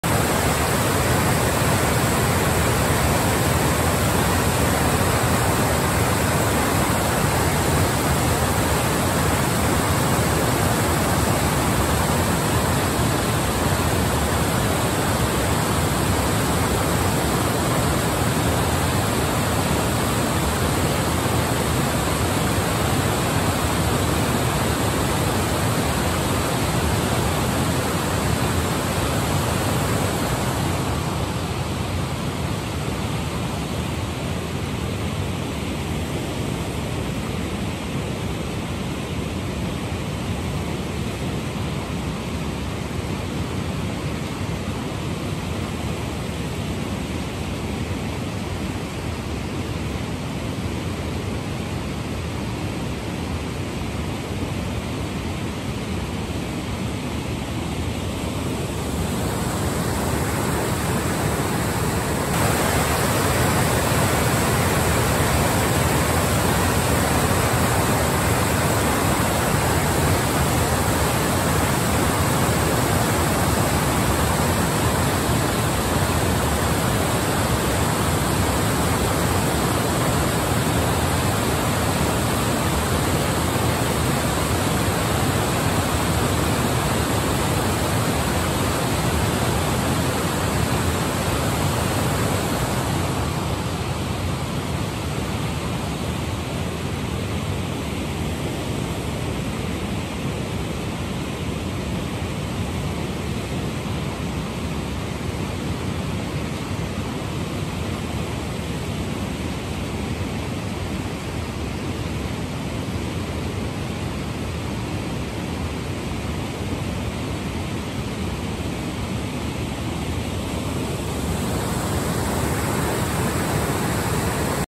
Lock-and-weir: don't be fooled by this minute version of Niagara (man-made) with a roaring sound
A lock built in 1912, with the weir upstream of the lock. Unfiltered sound of the infinitesimal power of water and woe befall any who misjudge or underestimate it